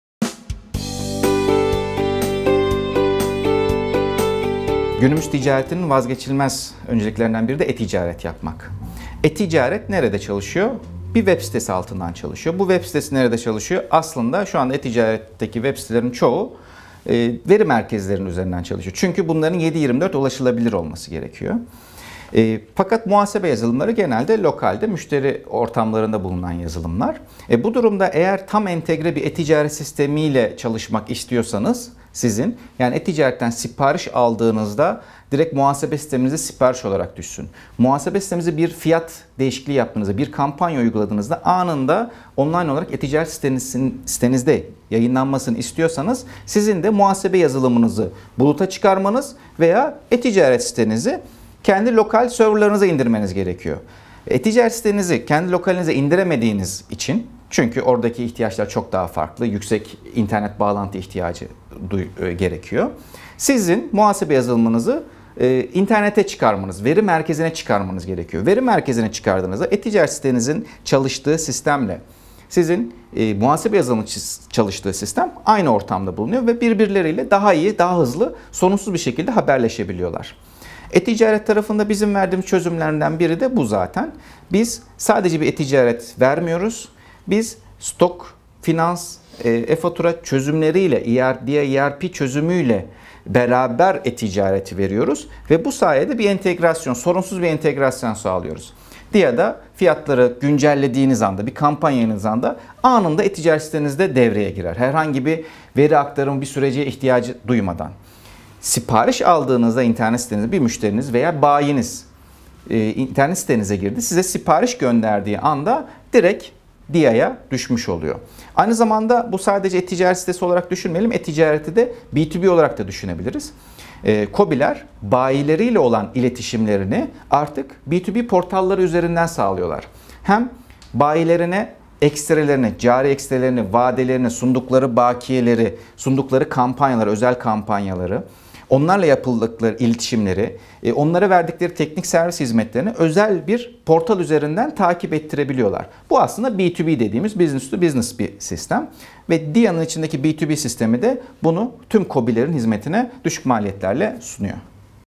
E-Ticaret ile bulut teknolojisi arasındaki ilişki nedir?
DİA Yazılım Genel Koordinatörü Suha Onay, e-ticaret ile bulut teknolojisi arasındaki ilişkiyi Wise TV aracılığıyla değerlendirdi.

"Günümüz ticaretinin vazgeçilmez önceliklerinden biri de e-ticaret yapmak." diyen Onay, "E-ticaretler bir web sitesi altından çalışıyor ve bu web sitelerinin çoğu da veri merkezleri üzerinden çalışıyor. Çünkü bunların çoğuna 7/24 ulaşılabilir olması gerekiyor. Fakat muhasebe yazılımları genelde lokalde müşteri ortamlarında bulunan yazılımlar. Bu durumda eğer tam entegre bir ticaret sitesi ile çalışmak istiyorsanız sizin e-ticaretten sipariş aldığınızda direk muhasebe sisteminize sipariş olarak düşsün. Muhasebe sisteminize fiyat değişikliği yaptığınızda bir kampanya uyguladığınızda anında online olarak ticaret sitesinde yayınlanmasını istiyorsanız sizin de muhasebe yazılımınızı buluta çıkarmanız veya e-ticaret sitenizi kendi lokal serverlerinize indirmeniz gerekiyor. Fakat kendi lokal serverlara indiremediği için yazılımlar veri merkezine yani buluta çıkmalı" şeklinde konuştu.